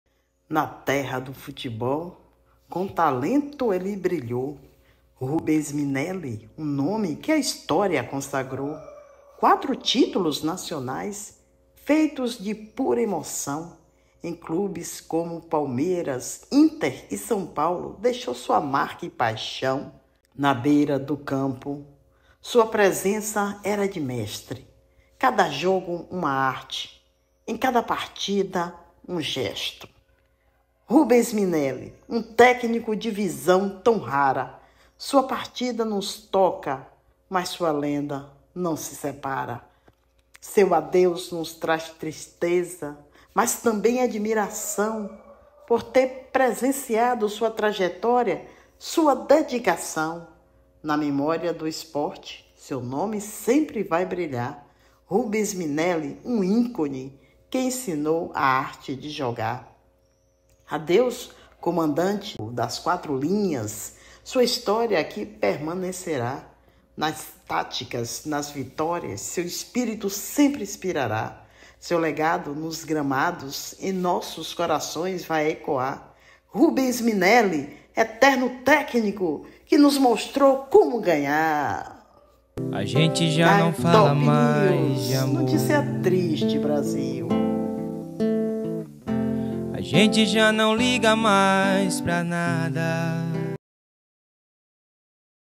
MORRE EX-TÉCNICO RUBENS MINELLI AOS 94 ANOS
Na terra do futebol, com talento ele brilhou,
Rubens Minelli, um nome que a história consagrou.
Quatro títulos nacionais, feitos de pura emoção,
Em clubes como Palmeiras, Inter e São Paulo, deixou sua marca e paixão.

Na beira do campo, sua presença era de mestre,
Cada jogo uma arte, em cada partida, um gesto.
Rubens Minelli, um técnico de visão tão rara,
Sua partida nos toca, mas sua lenda não se separa.

Seu adeus nos traz tristeza, mas também admiração,
Por ter presenciado sua trajetória, sua dedicação.
Na memória do esporte, seu nome sempre vai brilhar,
Rubens Minelli, um ícone, que ensinou a arte de jogar.

Adeus, comandante das quatro linhas, sua história aqui permanecerá,
Nas táticas, nas vitórias, seu espírito sempre inspirará.
Seu legado nos gramados, em nossos corações vai ecoar,
Rubens Minelli, eterno técnico, que nos mostrou como ganhar.